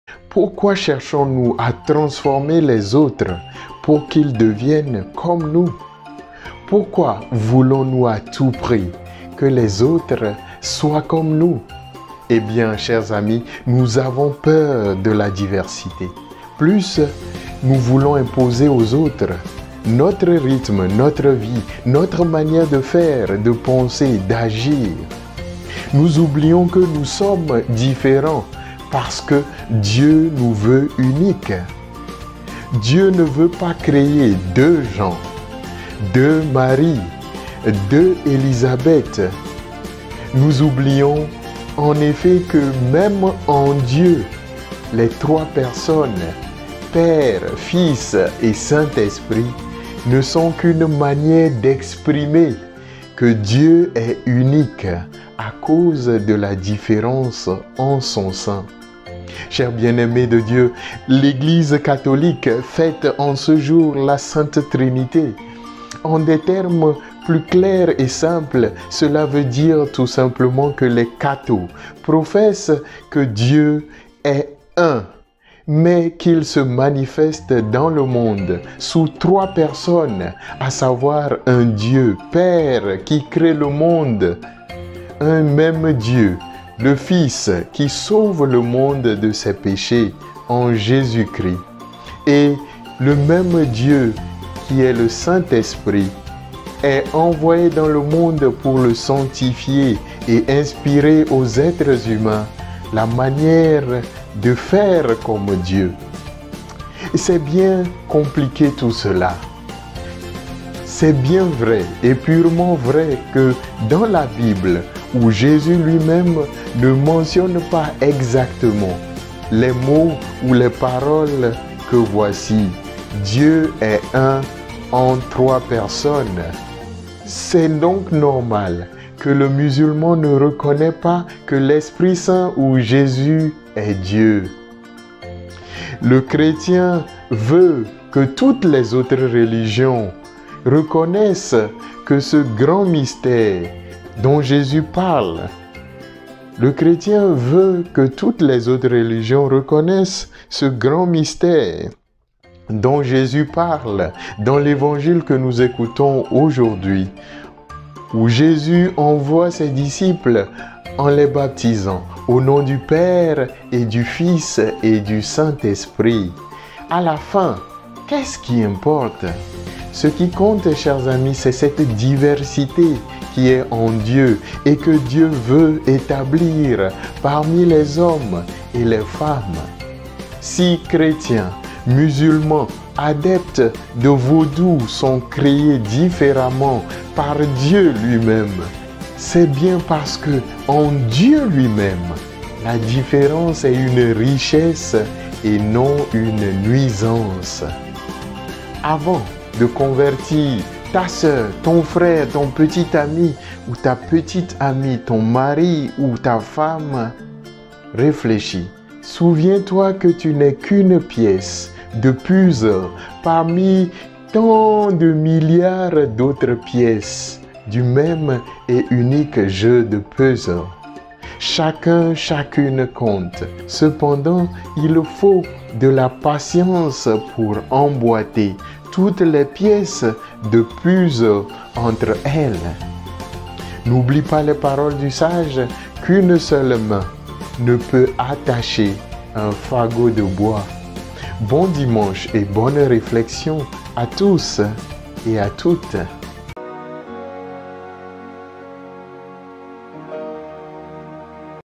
Dieu Un en trois personnes n'est pas dans la Bible
Méditation des textes liturgiques de la Solemnité de la Sainte Trinité